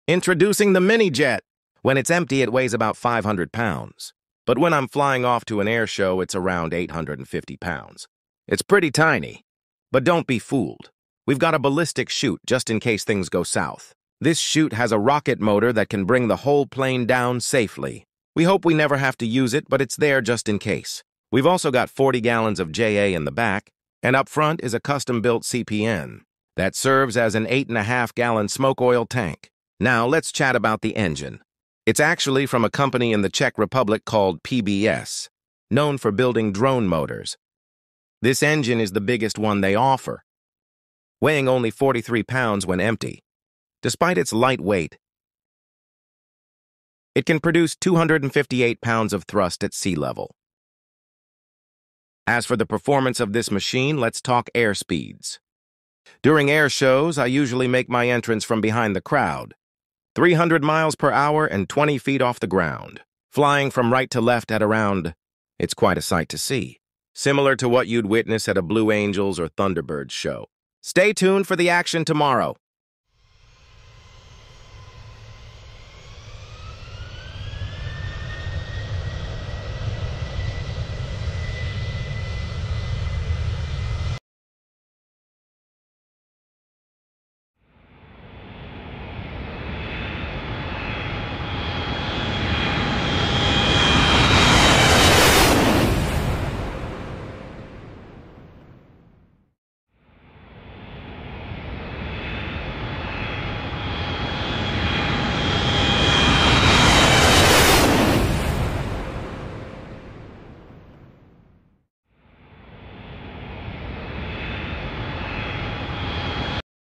SONNEX MINIJET🔥jetworld
MiniJet is a compact, high-performance personal jet designed for efficiency, speed, and versatility. Developed by Aveo Engineering, this aircraft features a sleek, aerodynamic design with modern avionics and lightweight composite materials, making it an attractive option for private pilots and aviation enthusiasts.

The MiniJet is powered by a single turbojet engine, providing impressive thrust and a cruise speed of around 250 knots (460 km/h). Despite its small size, the aircraft offers an exceptional power-to-weight ratio, allowing for agile maneuverability and a thrilling flying experience. Its short takeoff and landing (STOL) capabilities enable operations from small airstrips, making it ideal for personal travel and training.

The cockpit is designed with advanced digital avionics, ensuring ease of operation and improved situational awareness. The aircraft also incorporates fly-by-wire controls and modern safety features, making it both fun and safe to fly.

With a focus on affordability, fuel efficiency, and low maintenance costs, the Aveo MiniJet is a promising option for pilots looking for a cost-effective yet high-performance jet. Its compact size and high-speed capabilities make it a unique aircraft in the personal aviation market, offering an exhilarating yet practical flying experience.